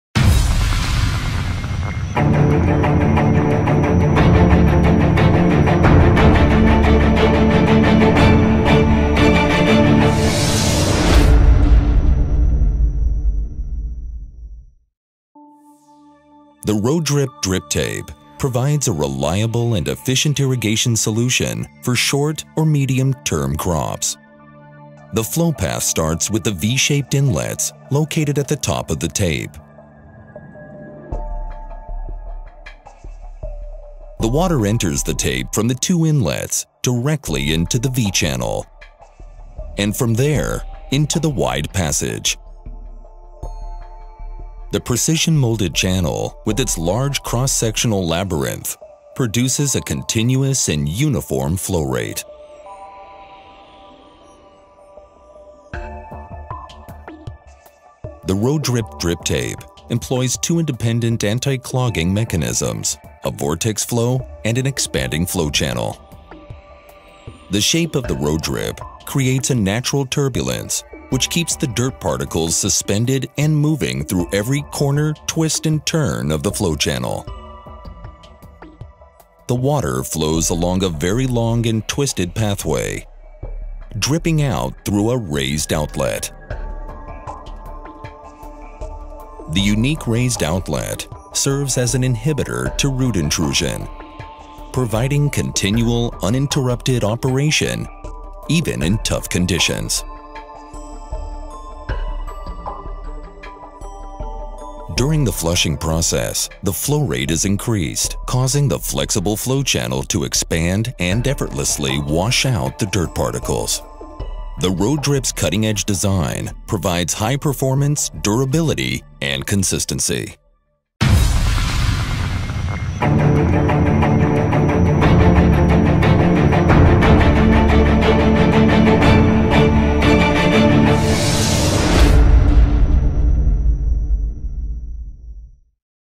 Irribiz - Rivulis Ro Drip
Ro-Drip Drip Tape provides a reliable, cost-effective solution for all your sub-surface and above ground drip irrigation needs. Ro-Drip is specifically engineered to deliver uniform flow rates while addressing the most difficult maintenance issue faced by drip growers today: blockage during high contaminant conditions.

Our patented Ro-Drip design employs two independent anti-plugging systems: Vortex Flow Action, which resists clogs by keeping particulate in suspension; and our exclusive Expanding Flow Channel, which reacts to blockages by actually changing shape, often purging even the toughest particulate build-up. Additionally, our patented manufacturing process creates a precision-molded flow channel with a large cross section, which can pass larger particulate and deliver the uniform flow rates you demand.